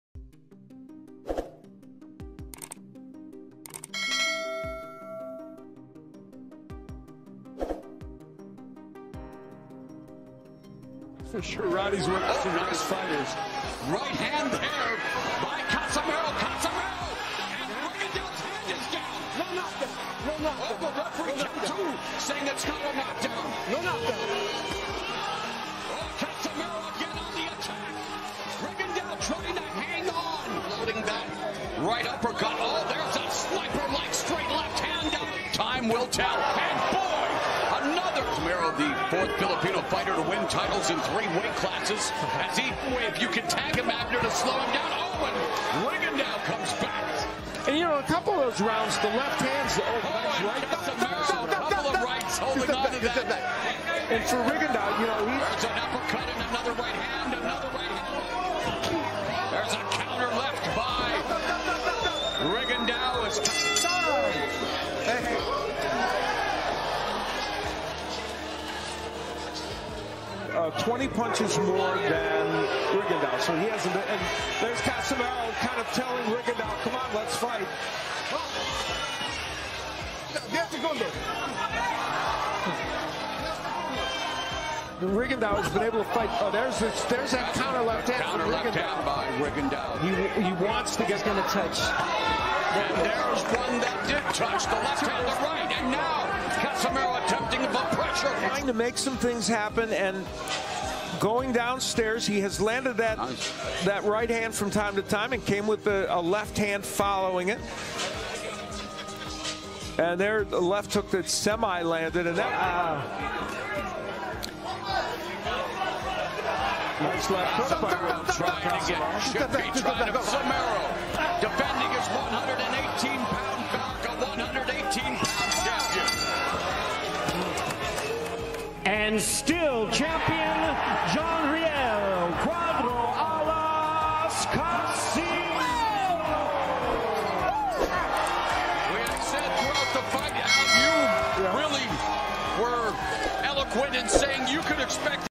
Casimero VS Rigondeaux Full Fight Highlights
John Riel Casimero improves to 31-4 and retains his WBO World Bantamweight Title following a split decision victory over Guillermo Rigondeaux in their 12-round bantamweight bout, live on SHOWTIME from Dignity Health Sports Park in Carson, CA.